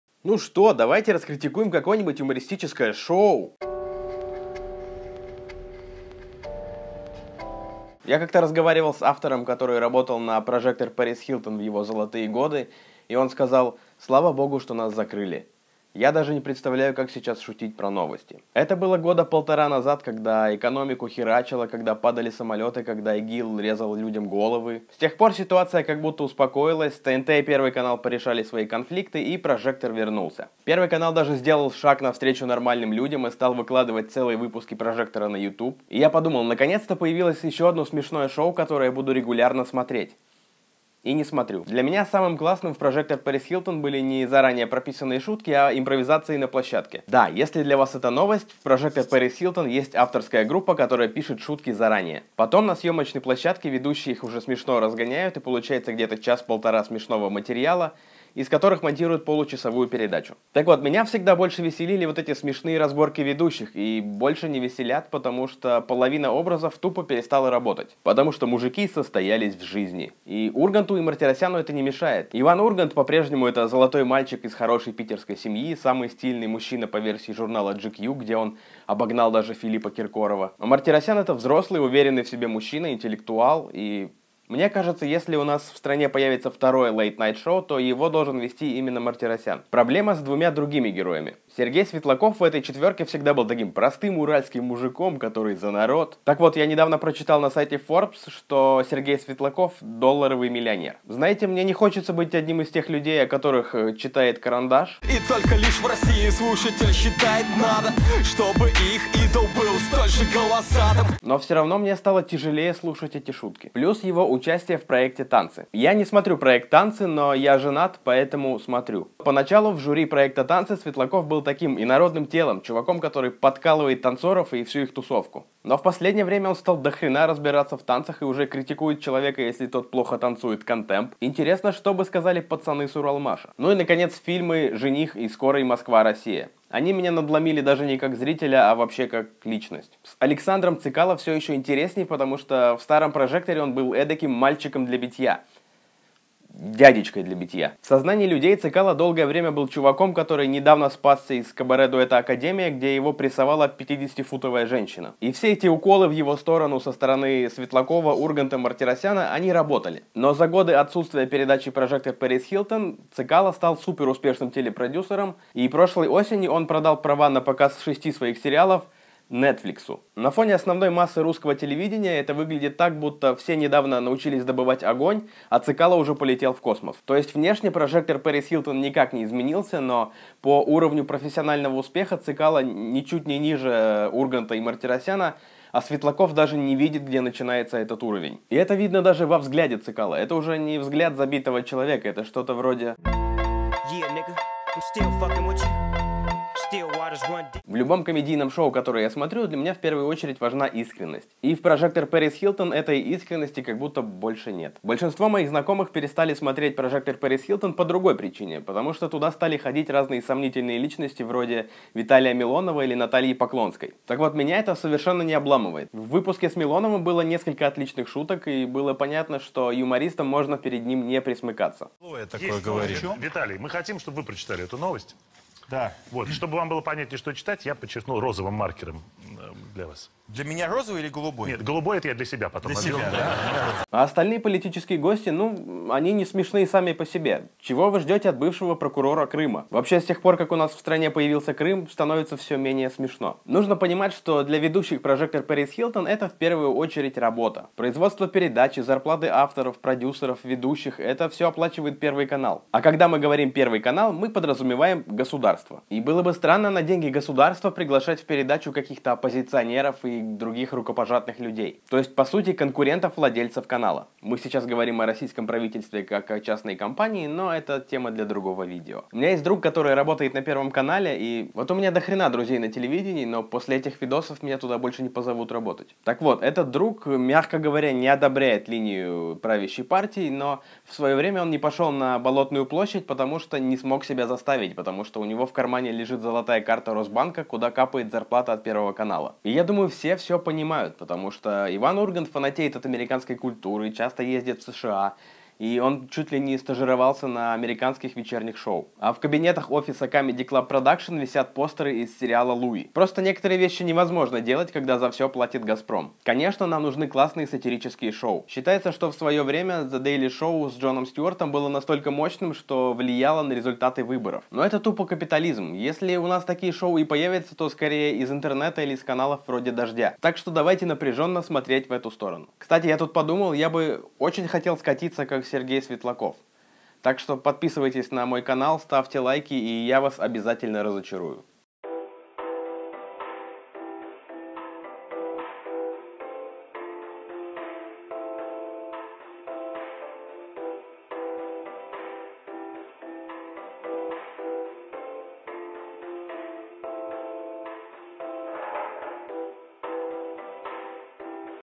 Почему новый «Прожекторперисхилтон» неинтересно смотреть | Денис Чужой
Моё самое большое разочарование 2017 года — возвращение «Прожекторперисхилтон». Зрители ждали ведущих в прежнем составе, и они это получили. Правда, никто не учёл, насколько Цекало и Светлаков изменились за время отсутствия ППХ. Первый стал суперсуспешным продюсером, и непонятно, как теперь над ним смеяться. Второй вместо рывка наверх осел в судейском кресле проекта «Танцы» и наснимал ужасных фильмов. И, в общем, над ним тоже смеяться тяжело.

P.S. Я по-прежнему уважаю всех, кто делает это шоу. Просто мне не нравится.